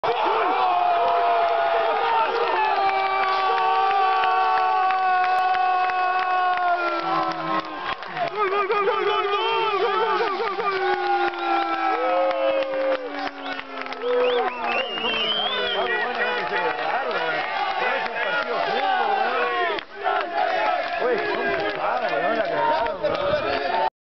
Magallanes 0-2 San Luis (Leonardo Olivera)
Tras un profundo pase, Leonardo Olivera le alcanza a pinchar la pelota a Hasbún sentenciando el 2-0 final.